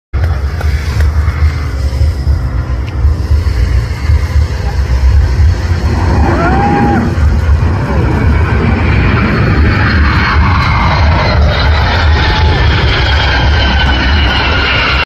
Amazing Low Fly By Nasa 747sp aka Sofia
Epic Fly by Sofia Final Display Before retirement.

make sure to check my whole video of this amazing aircraft.

Link for Sofia Full video display.